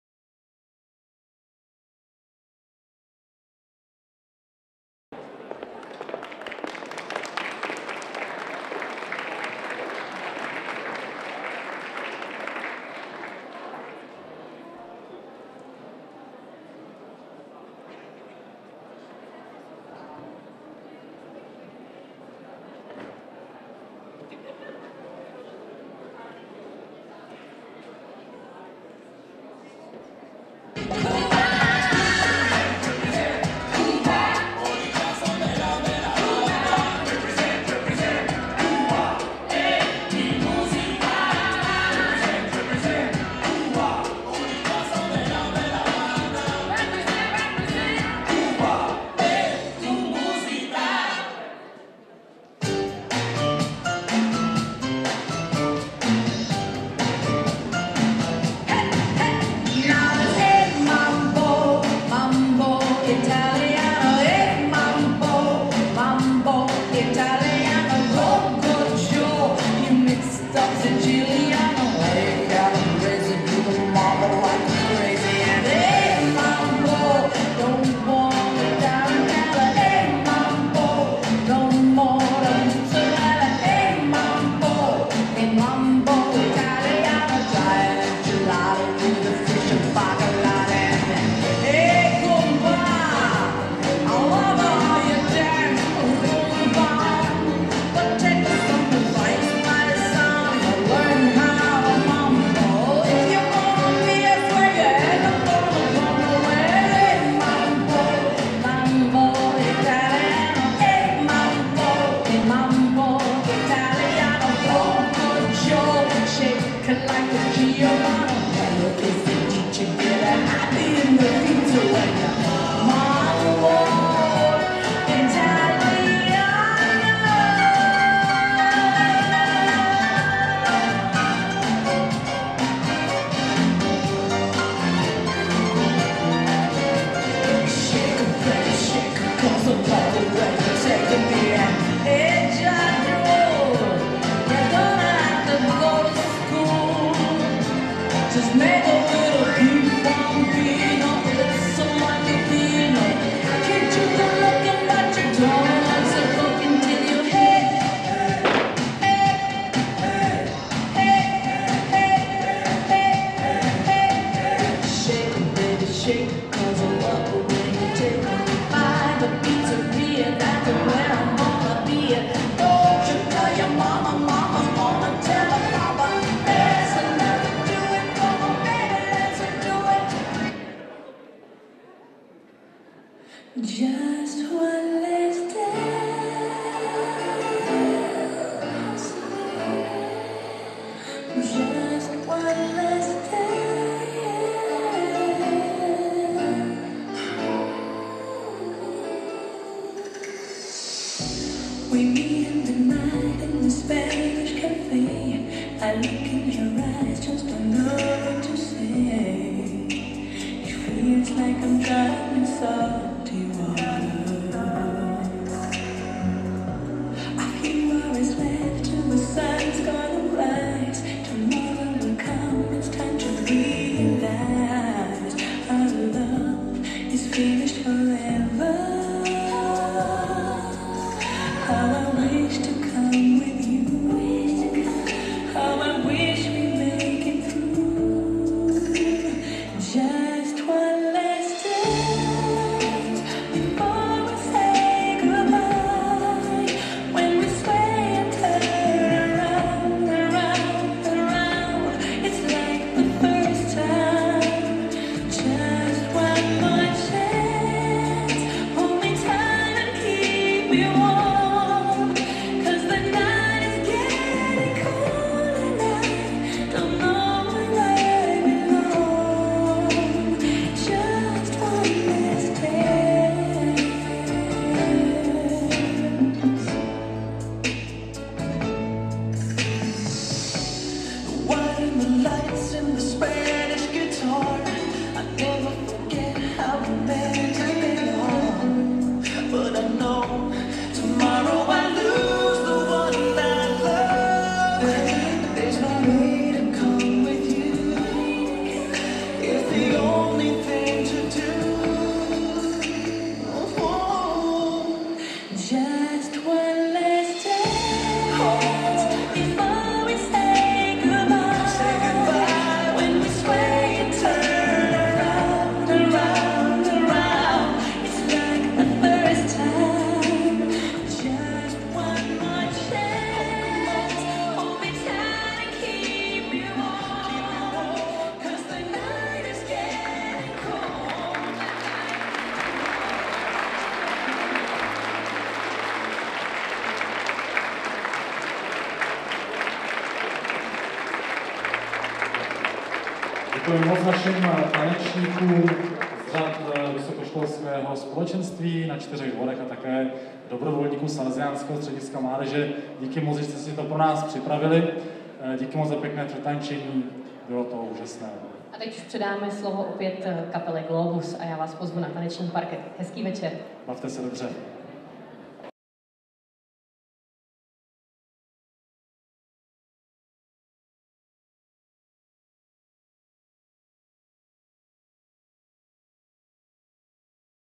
předtančení "JÁ A TA DRUHÁ"
Zahajovací tanec 24. Salesiánského plesu v Českých Budějovicích v podání mladých tanečníků společenství vysokoškoláků od sv. Vojtěcha.